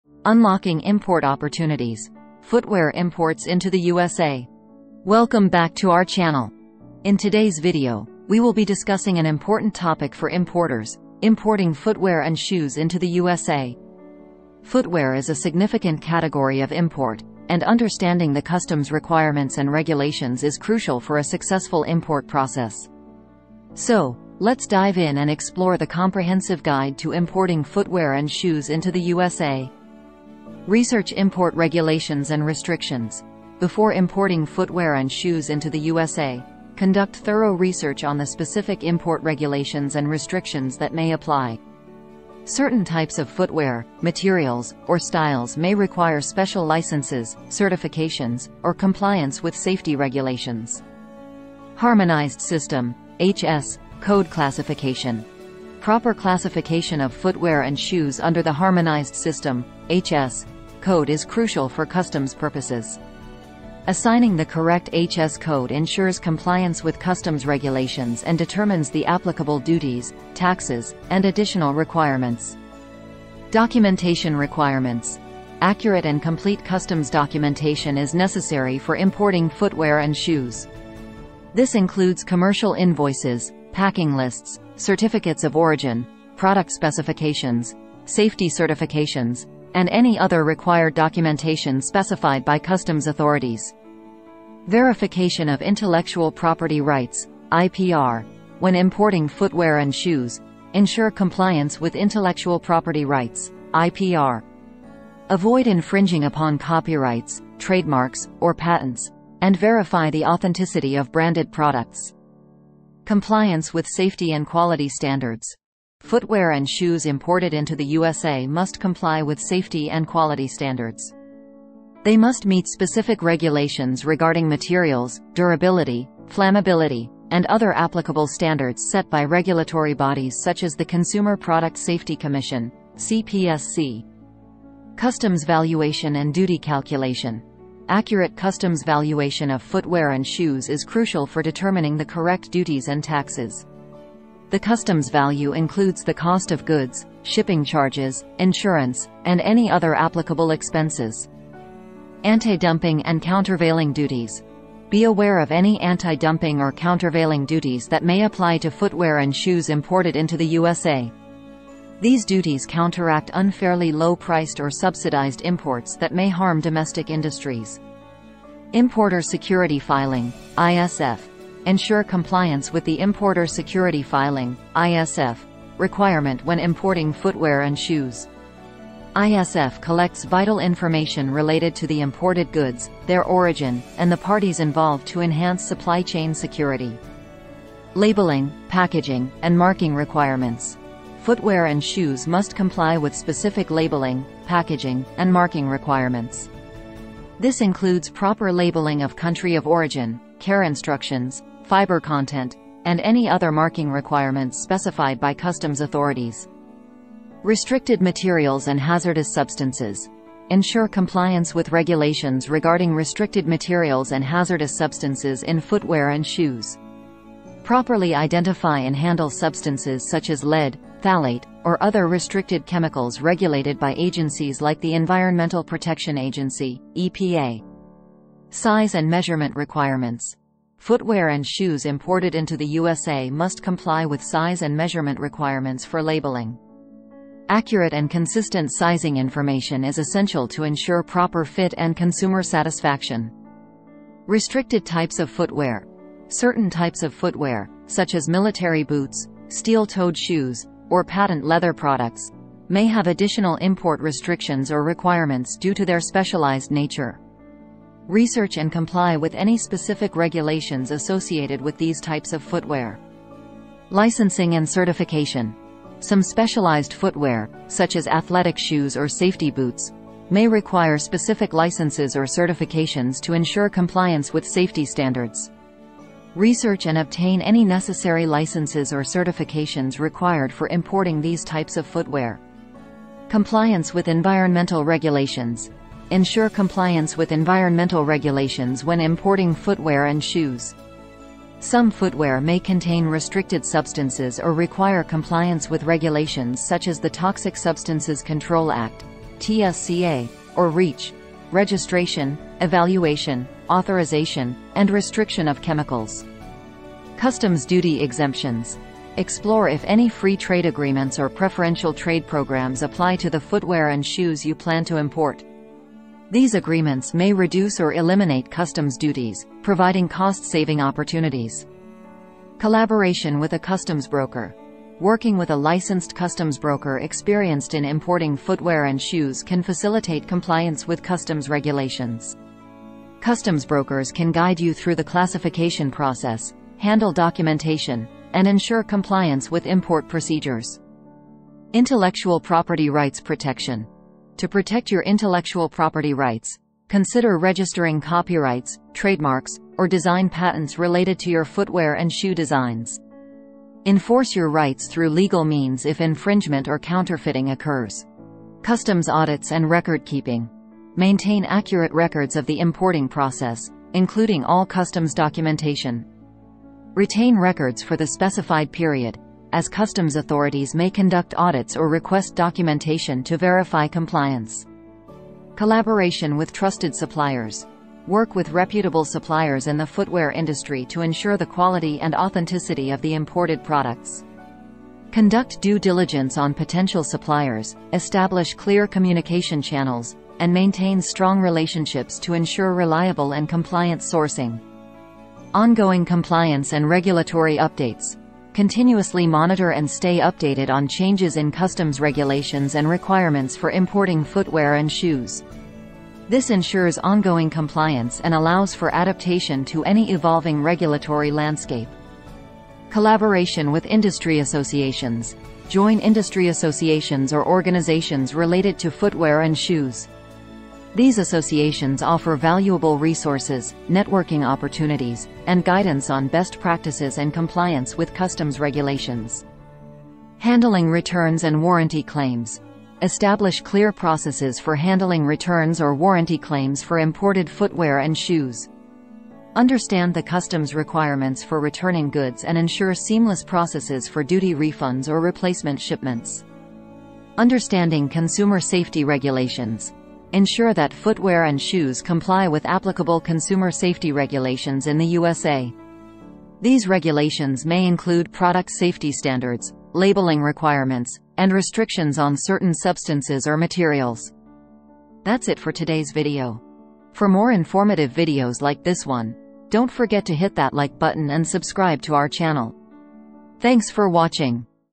"Unlocking Import Opportunities: Footwear Imports into the USA"
In this video, we offer a comprehensive guide to importing footwear and shoes into the USA, providing essential insights for businesses and individuals engaged in the footwear trade. Understanding the customs regulations, documentation requirements, and considerations specific to footwear imports is crucial for ensuring compliance and successful importation. Join us as we delve into the process of importing footwear, discuss key factors to consider, and provide practical tips for navigating customs clearance effectively.

Video Disclaimer Here: This educational content is unassociated with US governmental bodies.